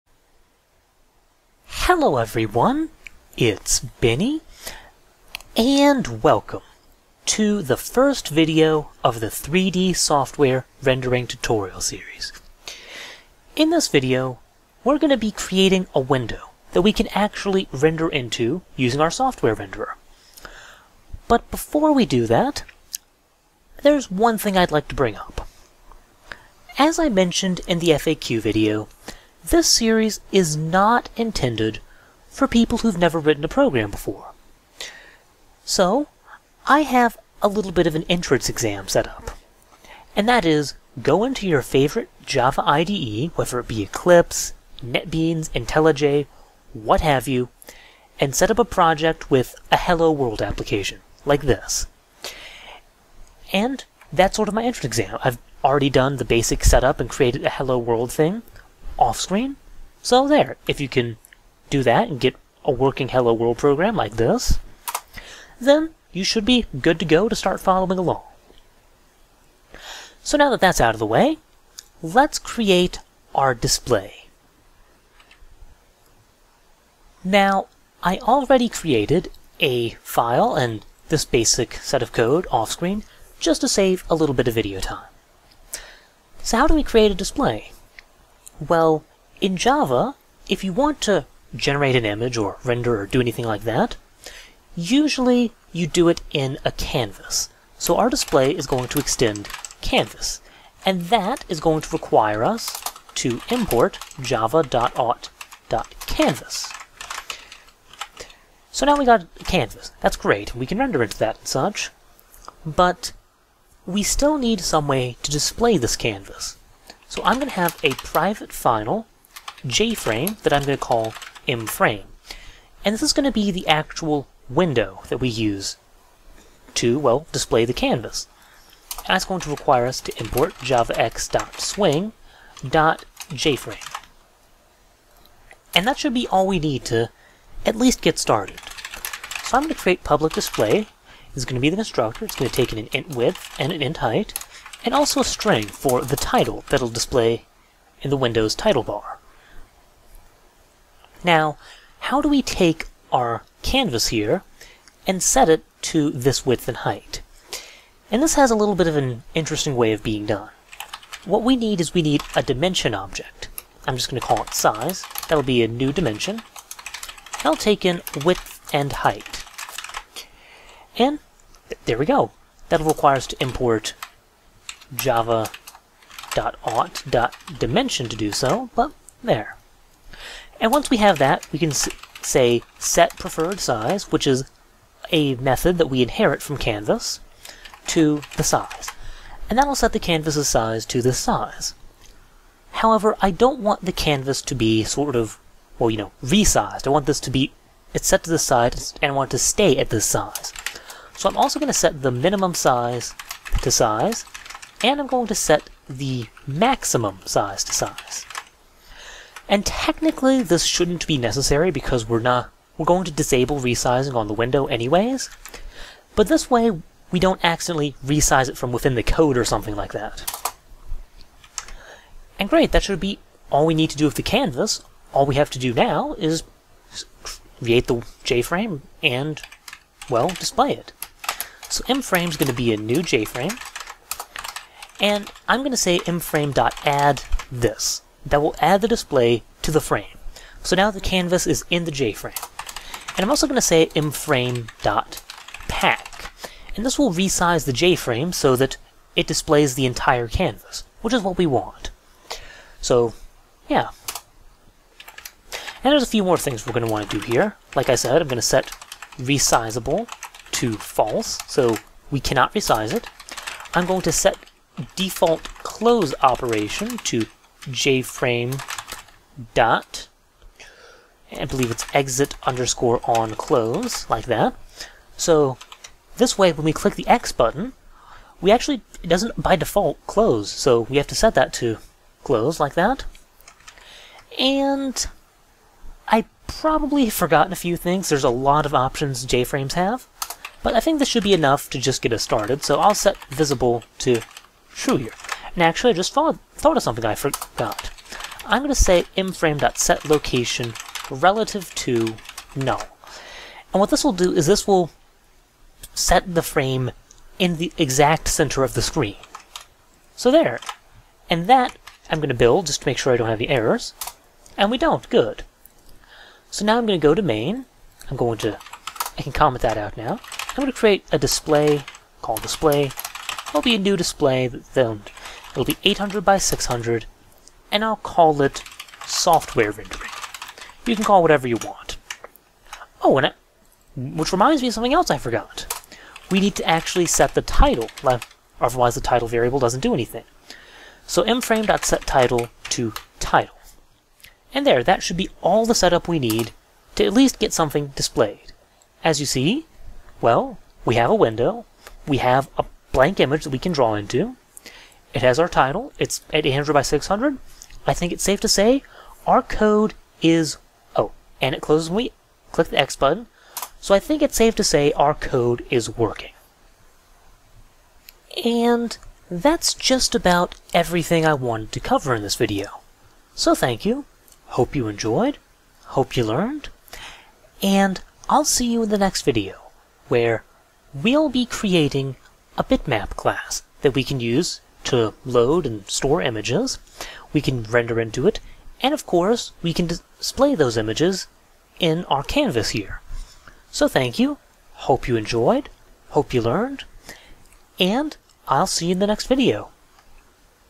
#1 3D Software Rendering Tutorial: Creating a Display
In this video, we create a display that can present our software rendered images.